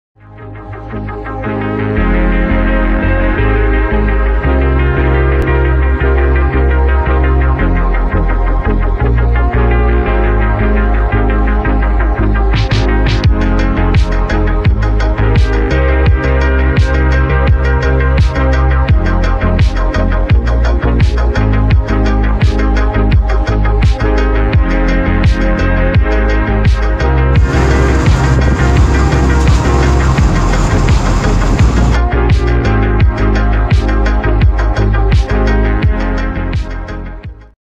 Teaser - Campione Invictus HD da Catedral
Novidades? Sim senhor! Em breve vocês irão conhecer a mais nova aquisição da Catedral Turismo para o serviço rodoviário: dessa vez não é New G7, mas vem mais aí! A bola da vez é um Campione Invictus HD adquirido recentemente pela empresa.
Há rumores de que tenha sido adquirido para testes.